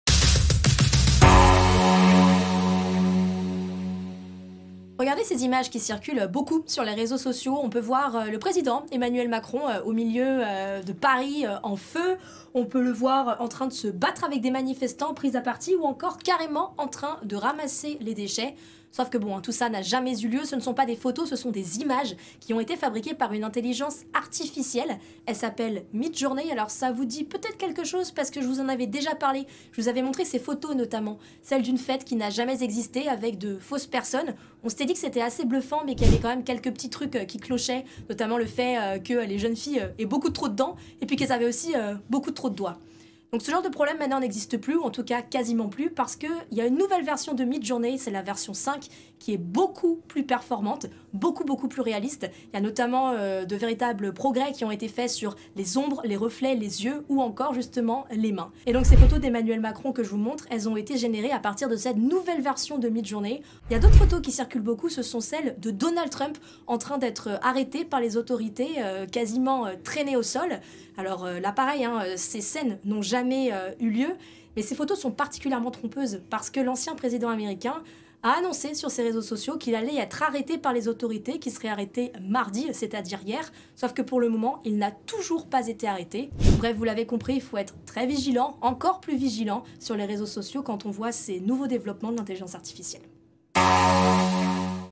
Emmanuel Macron dans les poubelles grâce à une intelligence artificielle
Grâce à l'intelligence artificielle Midjourney, plusieurs personnalités politique se sont retrouvées dans des situations assez insolites, comme Emmanuel Macron ou Donald Trump.